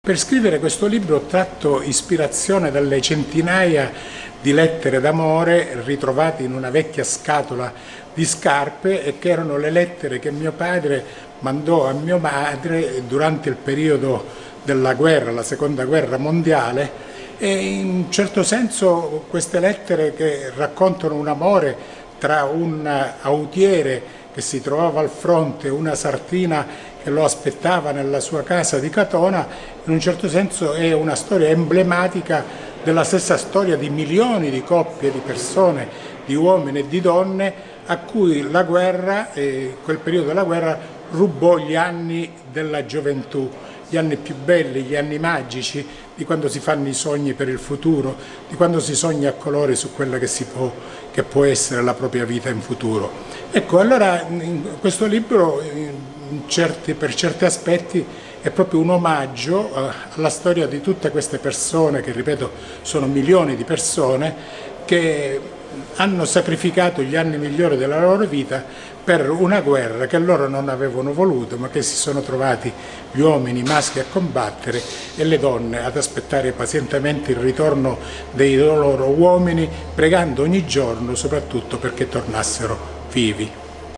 Presentato a Bagnara Calabra il libro di Mimmo Nunnari "Guerra e amore nell'Italia di Mussolini'
"Le donne, i cavalier, l'arme, gli amori".
Così forse avrebbe replicato Ariosto per entrare nei temi dell' ultima fatica letteraria del giornalista Domenico Nunnari "Guerra e amore nell'Italia di Mussolini", presentata ieri all' interno dell' aula consiliare di Palazzo San Nicola a Bagnara Calabra. Sì, perchè all' interno della narrazione degli anni della seconda Guerra Mondiale si intreccia la storia, contemporaneamente struggente e dolce, di due innamorati a cui il il conflitto ha sottratto gli anni migliori, gli anni della spensieratezza della giovinezza. Nunnari infatti, per la scrittura del libro si è ispirato alle numerose lettere che l' autiere Giuseppe Nunnari scriveva dal fronte di battaglia dell' Africa del Nord e poi dalla prigionia in Sud Africa e Inghilterra, alla fidanzata Domenica Barberi. "Un libro urgente e necessario, che dovevo ai miei genitori" - spiega l' autore.
Una vicenda che insegna che senza l' amore, ogni altro valore perderebbe la sua originale configurazione, che aiuta a interrogarsi seriamente sullo sfaldamento dei valori fondamentali che oggi interessa un quadro sociale piuttosto compromesso.
Dalla presentazione, moderata dalla Prof.ssa Macrì e arricchita dagli interventi dell' Avv. Latella e dal Magistrato Ramondino, è anche emerso il senso di attaccamento che l'autore ha con Bagnara, sottolineando che anche da un piccolo paese di Calabria possano nascere e fiorire importanti occasioni.